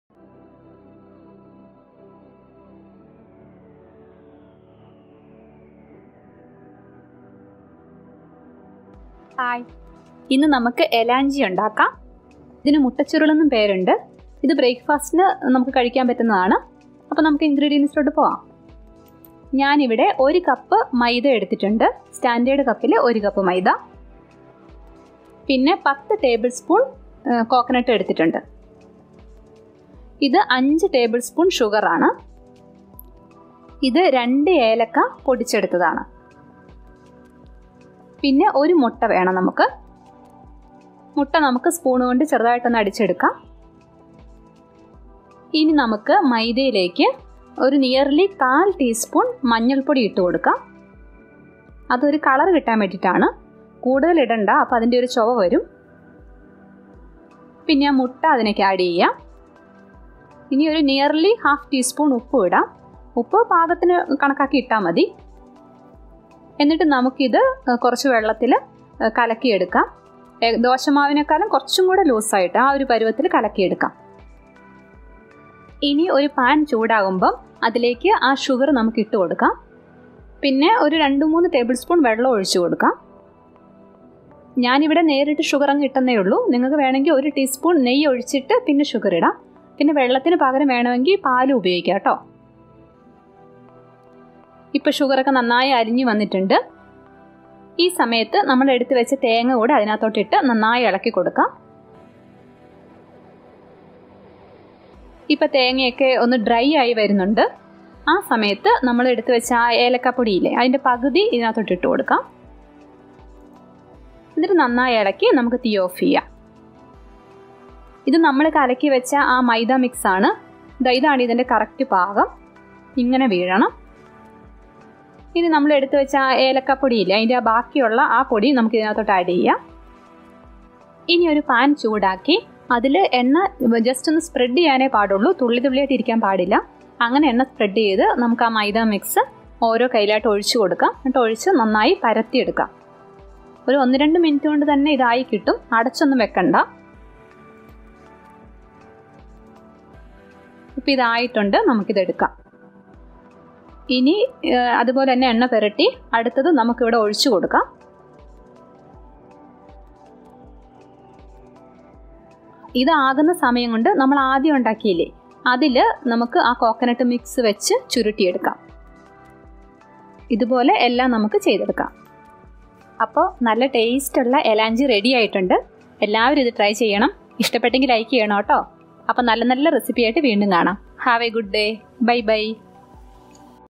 ELANCHI RECIPE/ MUTTA CHURUL /EGG ROLL/MALABAR DISH / MALAYALAM/BREAKFAST RECIPE/MUTTA KUZHALAPPAM
INGREDIENTS:
1 CUP -MAIDA
10 TB S - COCONUT
5 TB S - SUGAR
2  -CARDAMOM

 EGG